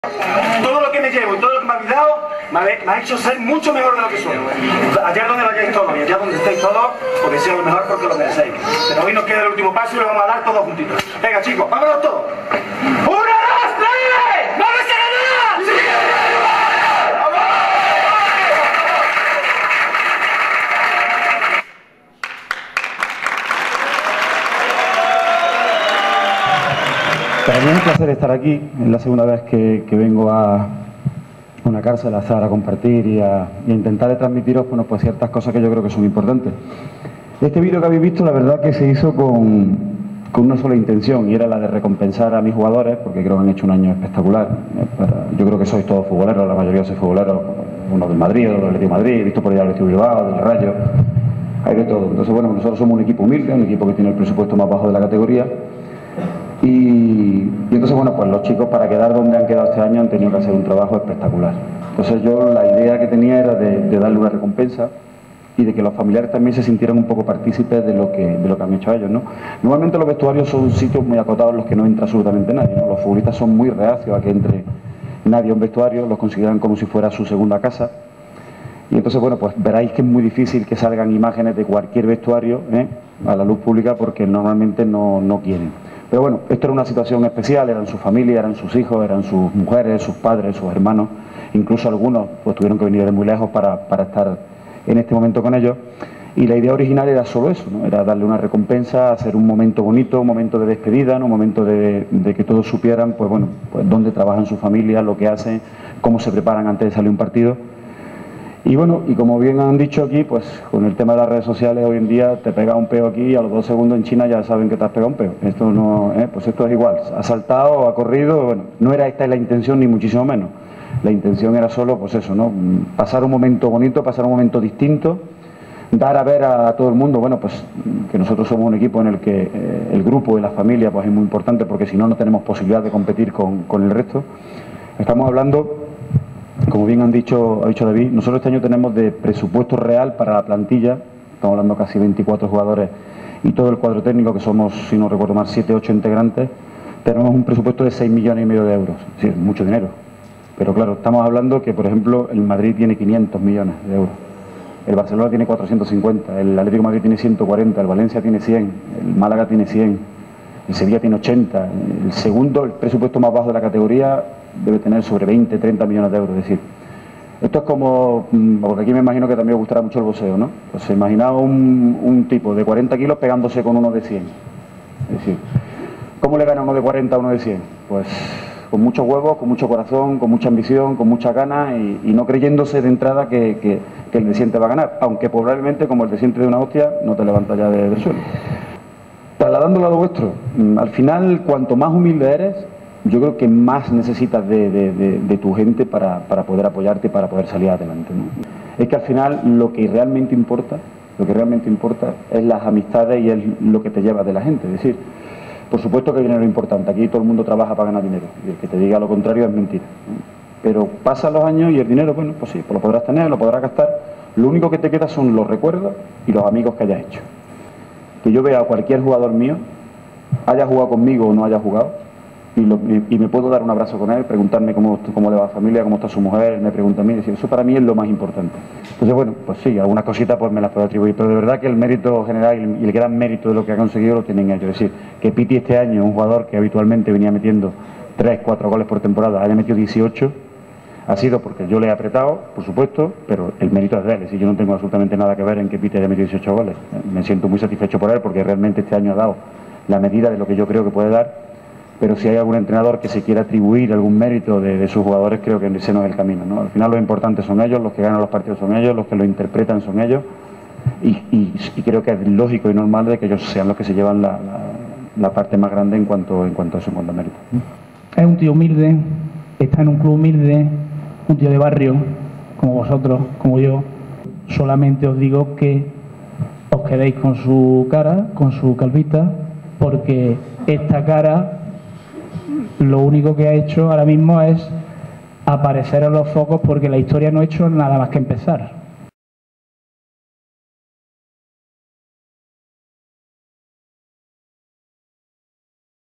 Charla de Paco Jémez en el Centro Penitenciario Madrid VI
Charla de Paco Jémez, entrenador del Rayo Vallecano, en el Centro Penitenciario Madrid VI de Aranjuez.